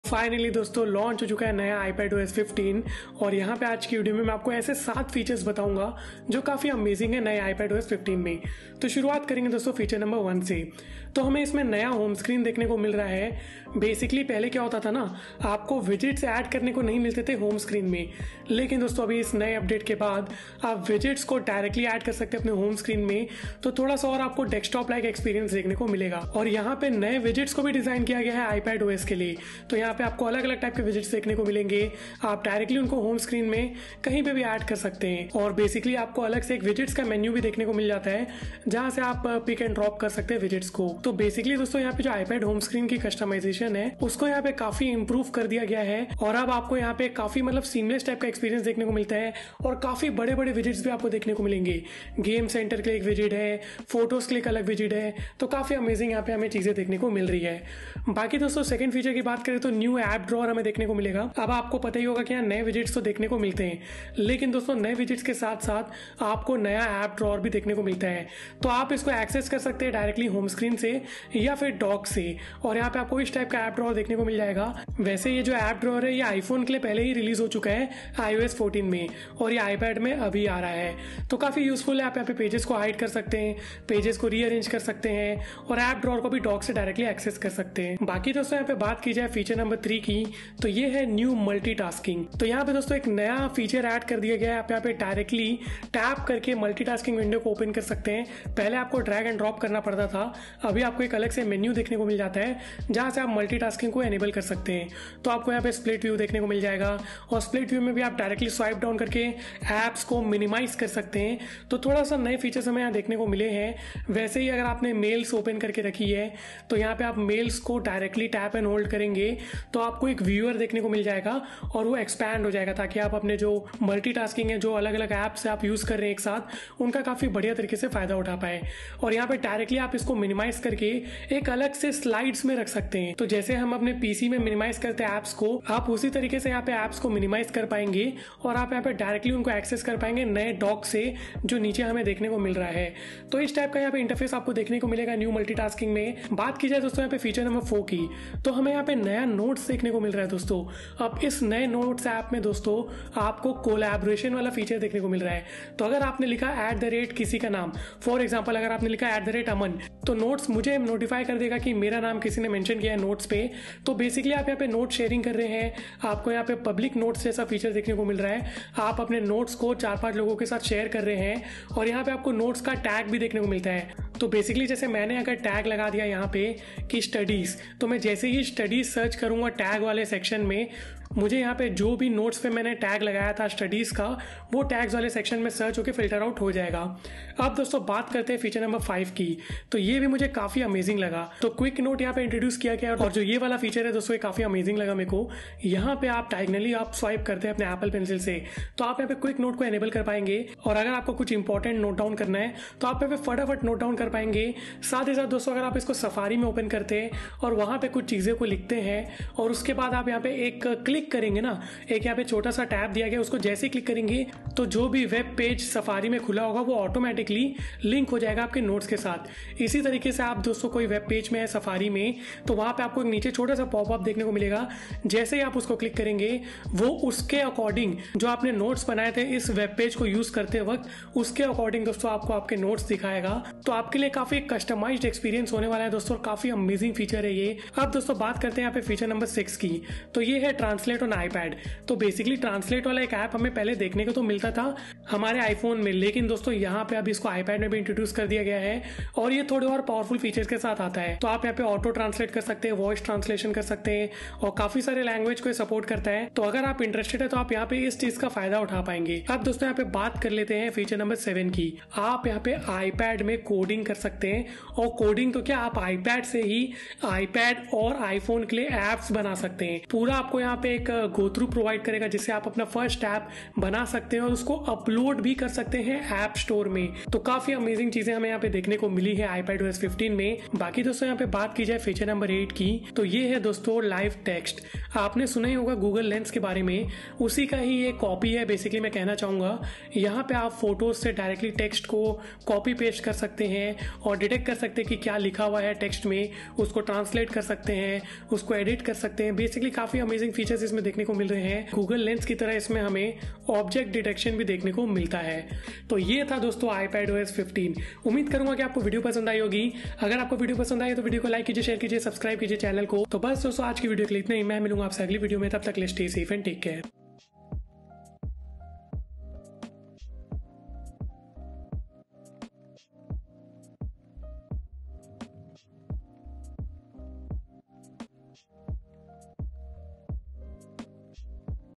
iPadOS / iOS 15 Full Review | Top 8 Features of iPadOS 15 in Hindi
In this video we are going to talk about the latest iPad OS 15 that has been launched by apple in WWDC 2021 Event.